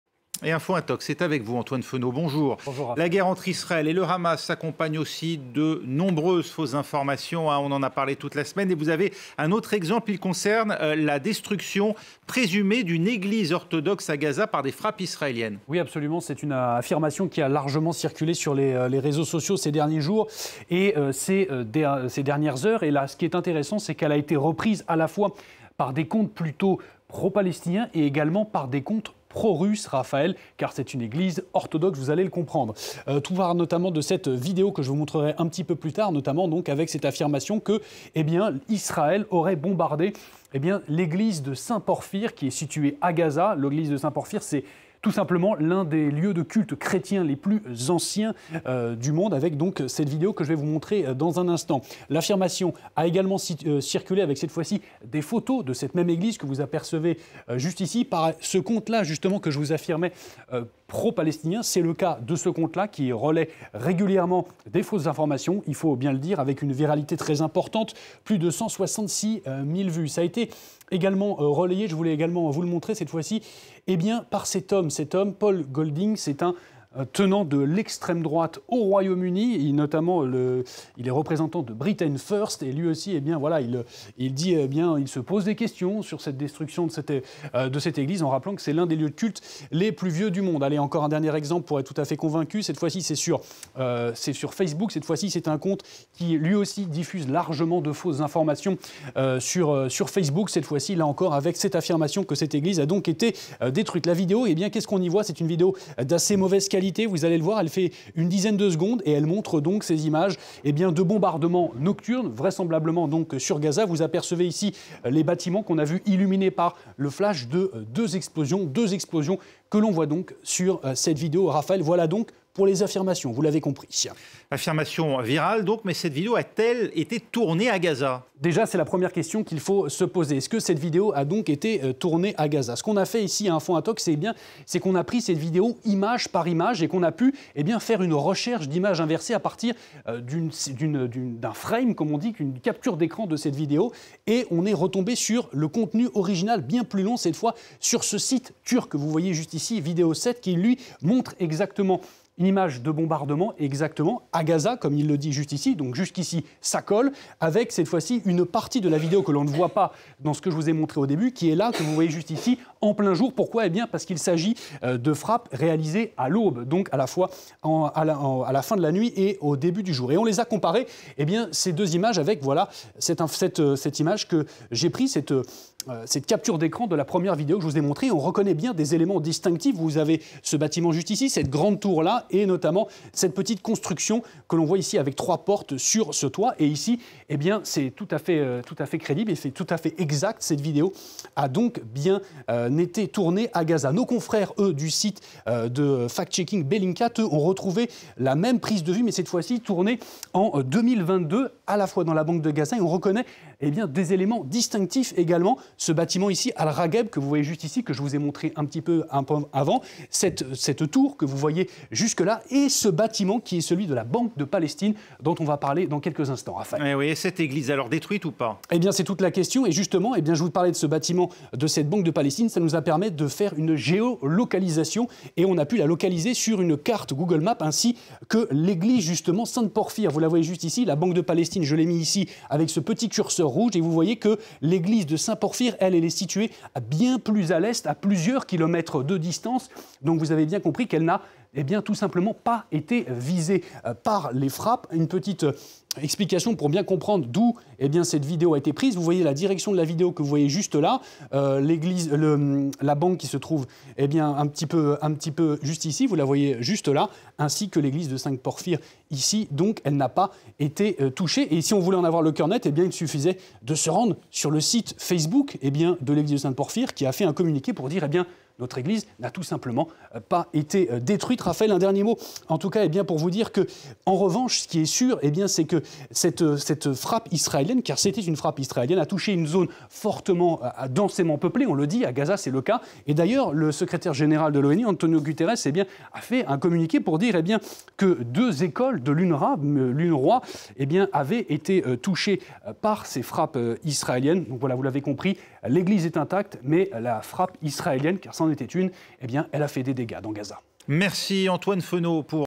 Une Eglise orthodoxe détruite par des frappes israéliennes à Gaza? Prudence ! • FRANCE 24
Une église orthodoxe située en plein cœur de Gaza aurait été détruite par des frappes de l'Etat hébreu. Affirmation reprise par des comptes pro-palestiniens mais aussi pro-russes. Alors ce lieu de culte a-t-il été détruit? Explications.